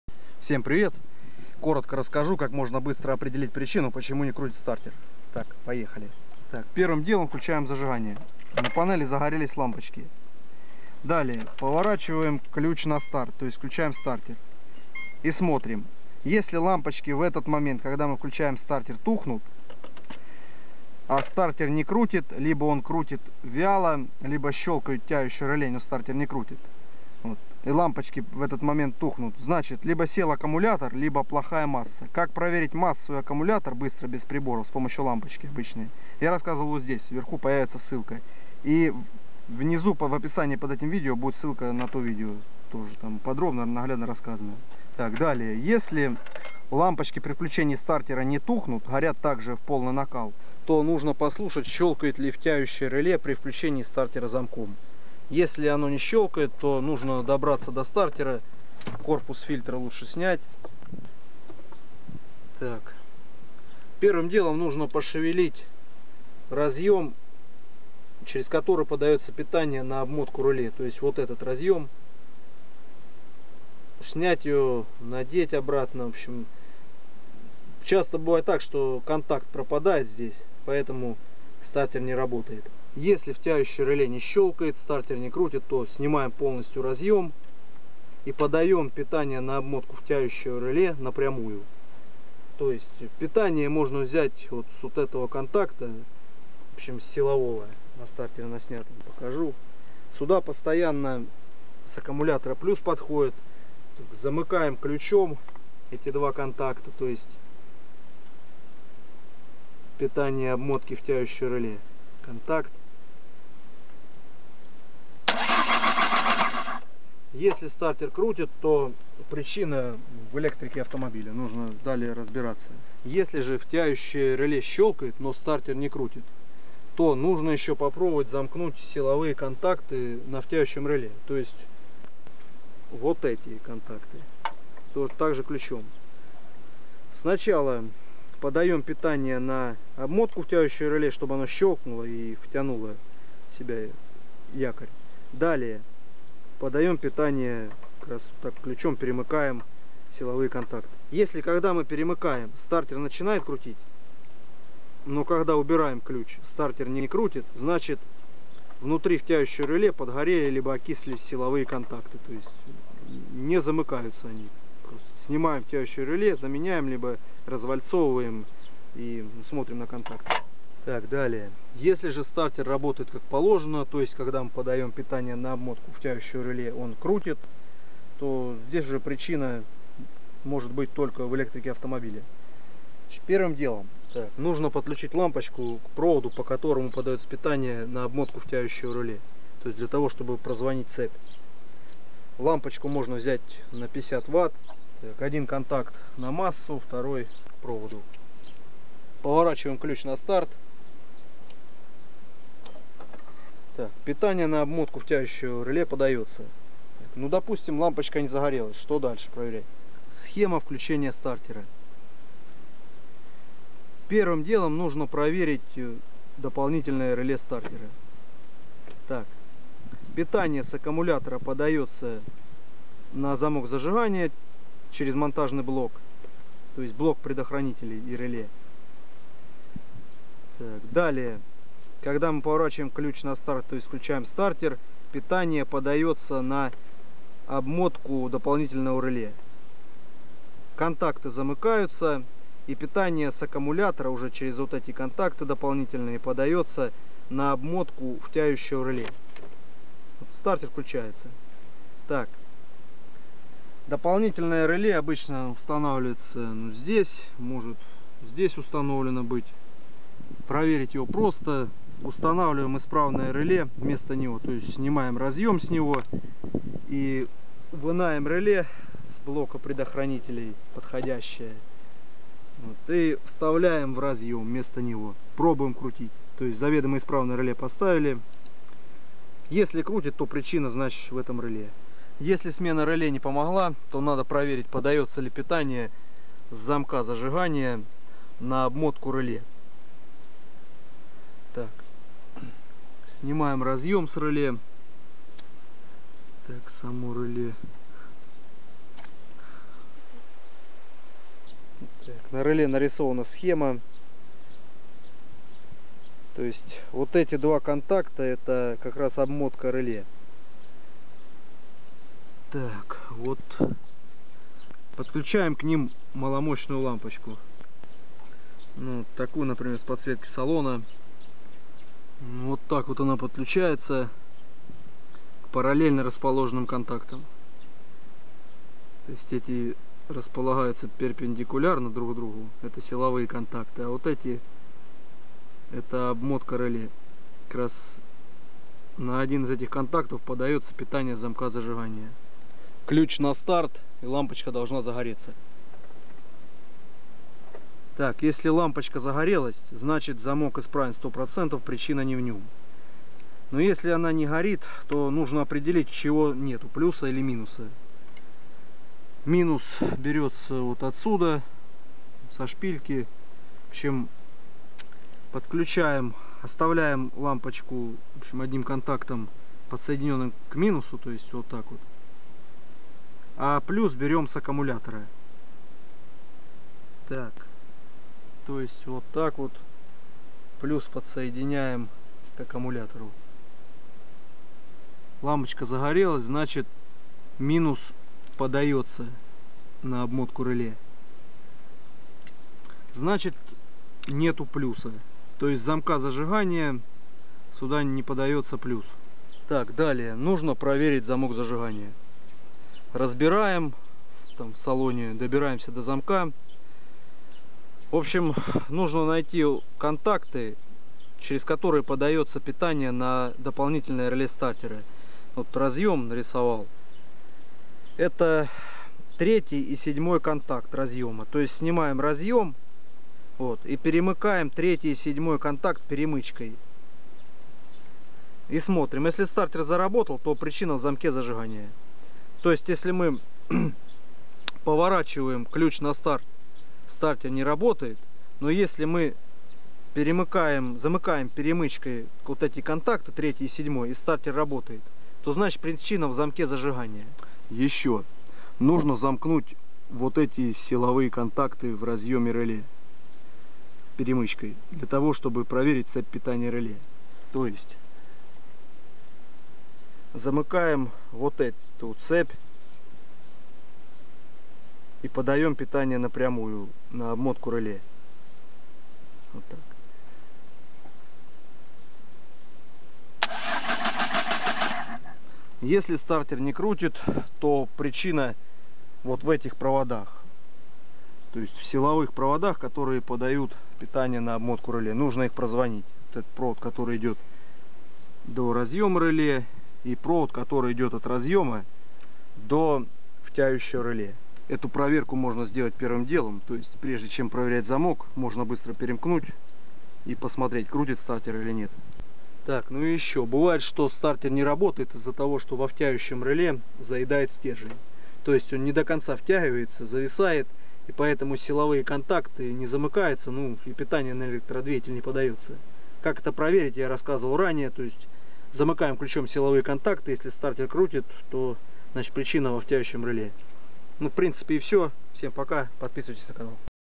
Не крутит стартер. Поиск причины
Если не крутит стартер на автомобиле, это видео поможет вам найти причину. В видео рассказано как проверить стартер на авто не снимая, как проверить дополнительное реле стартера, как проверить замок зажигания, как прозвонить цепь питания стартера. Проверка втягивающего реле. Поиск причины почему не крутит стартер показан на примере ваз 2114.